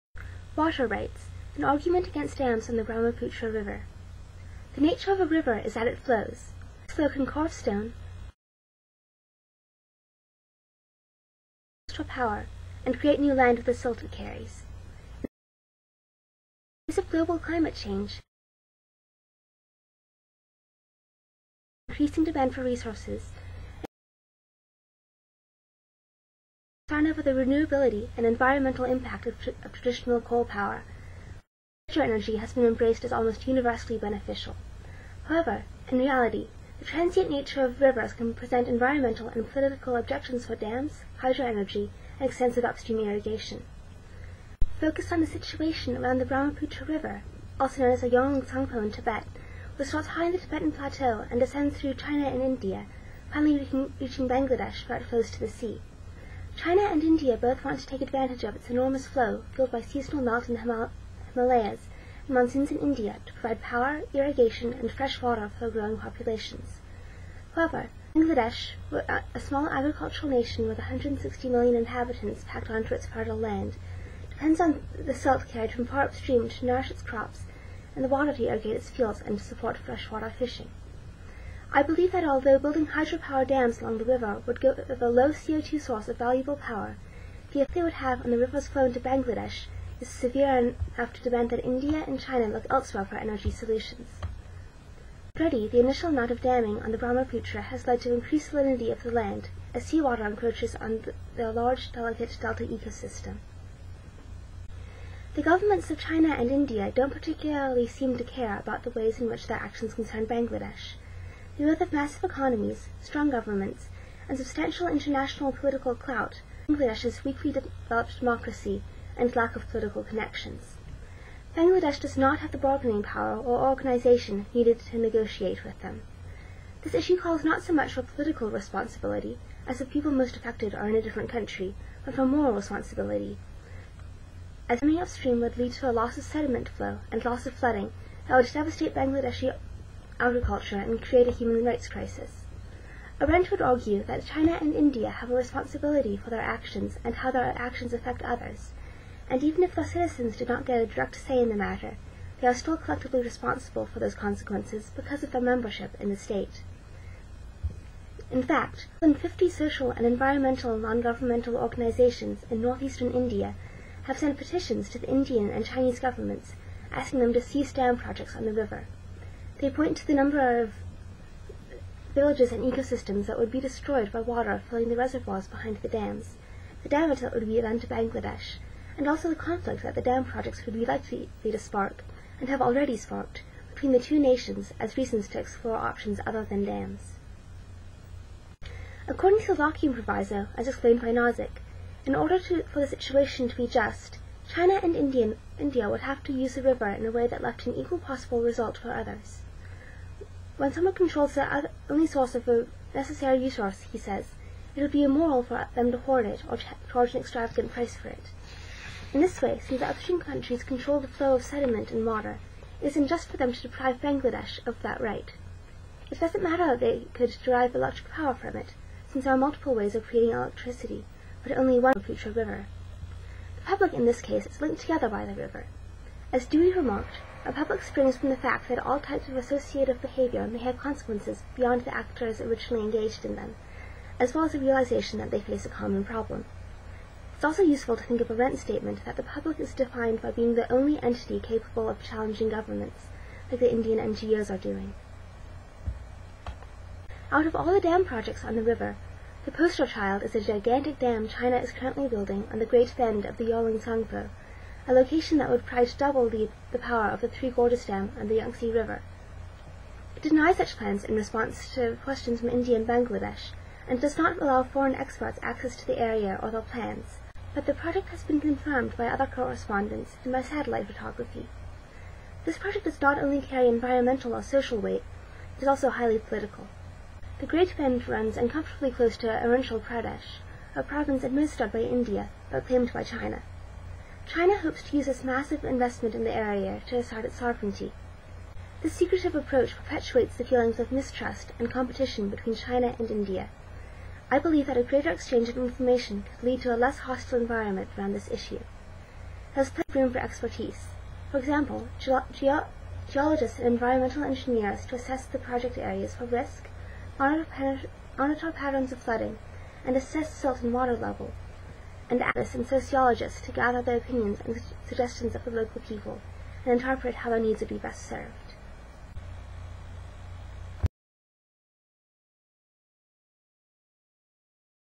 Water Rights: an argument against damming the Brahmaputra River
My PSCI 253 Ethics and Policy project, regrettably recorded at 4:00 am. There are some skips in the first 20 seconds of the video: here's the text.
The nature of a river is that it flows; this flow can carve stone, be harnessed for power, and create new land with the silt it carries. In the face of global climate change, increasing demand for resources, and concern over the renewability and environmental impact of traditional coal power, hydro-energy has been embraced as almost universally beneficial. However in reality, the transient nature of rivers can present environmental and political objections for dams, hydro-energy and extensive upstream irrigation.